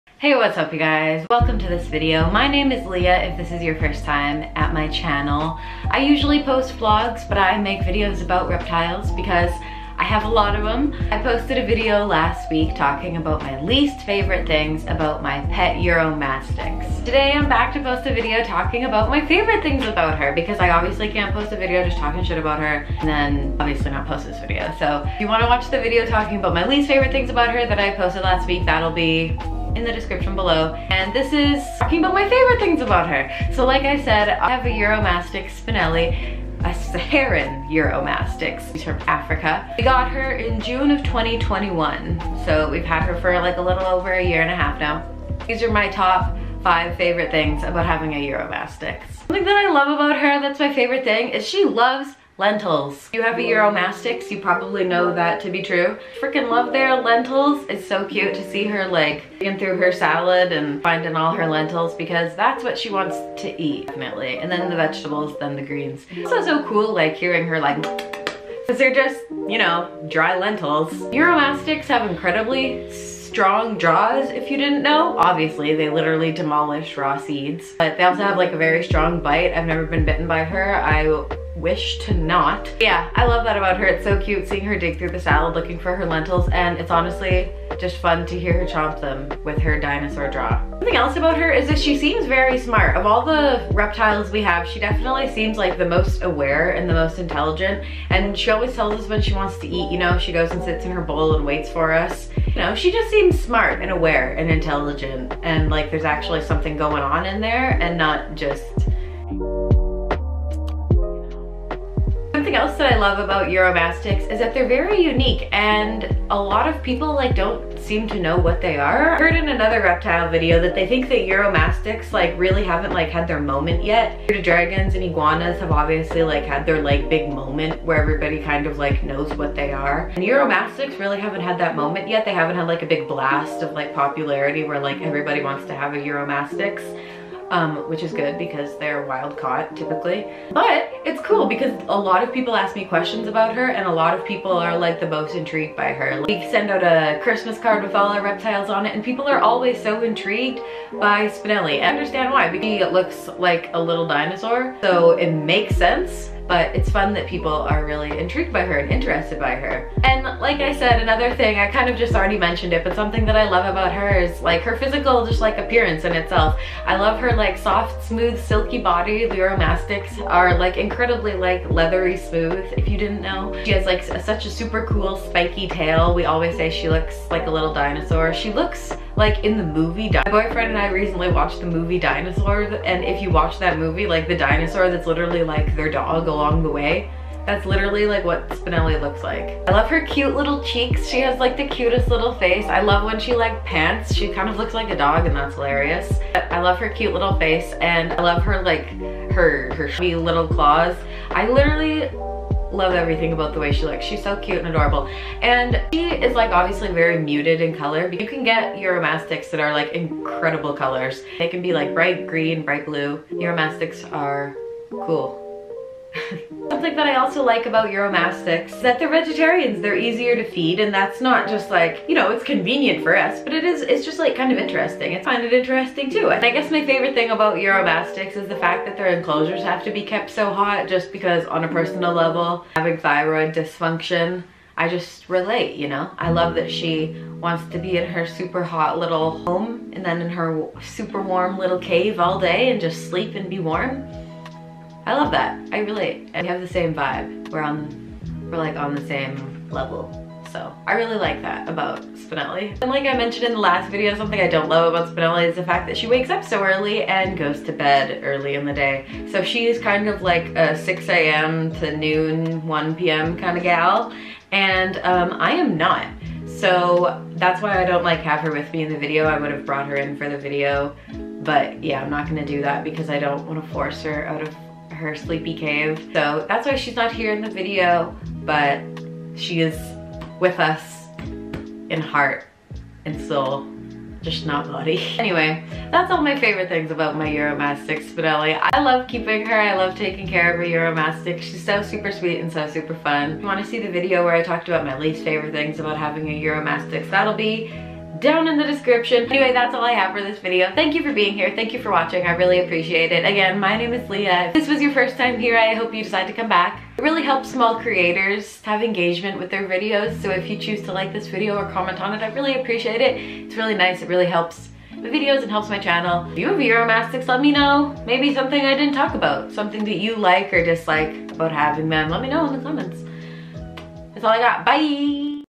5 of my FAVOURITE THINGS about my UROMASTYX
Last week i posted a video talking about my least favourite things about our uromastyx Spinelli. This is the second video, talking about my favourite things about her.